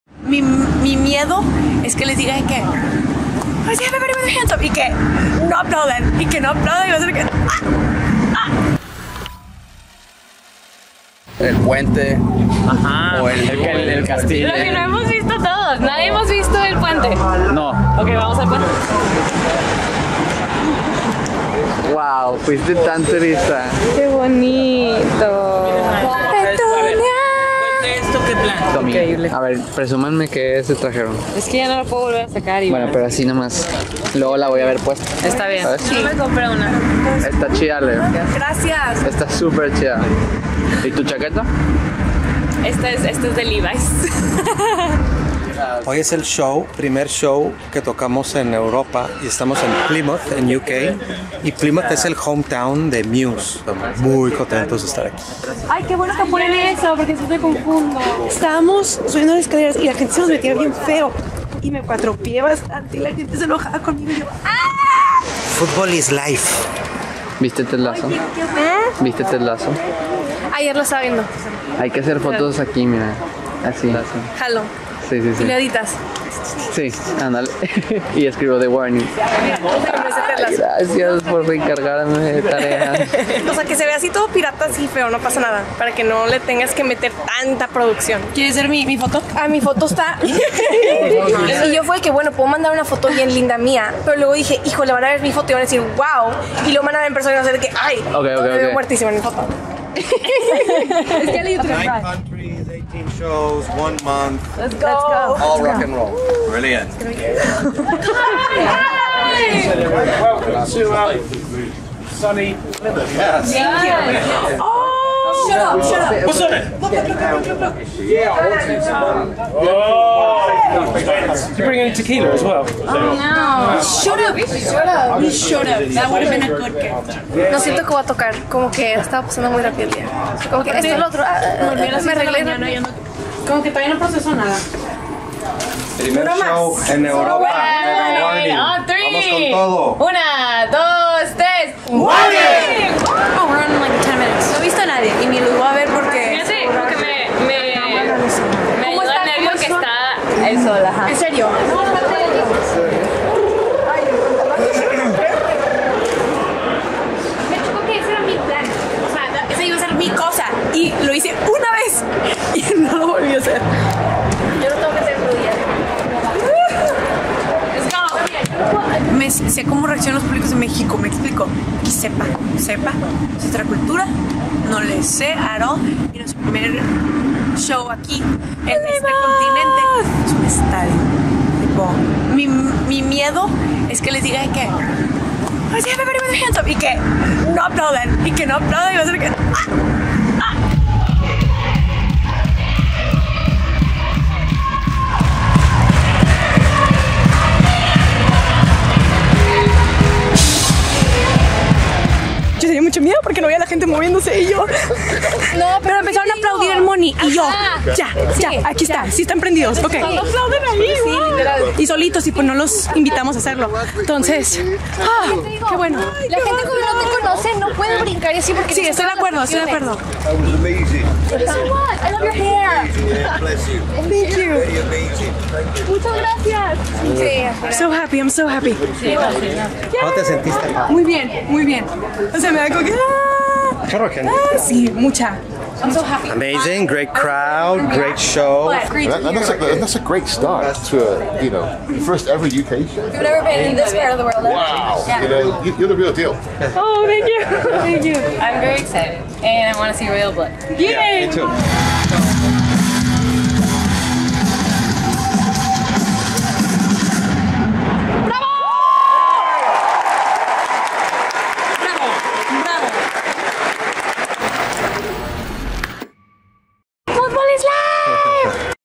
The Warning - Plymouth, UK Supporting Muse Aftermovie
The Warning: Three Sisters from Monterrey, México, Daniela (Lead Vocal, Guitar), Paulina (Drummer, Background Vocals), and Alejandra Villarreal (Bass, Background Vocals).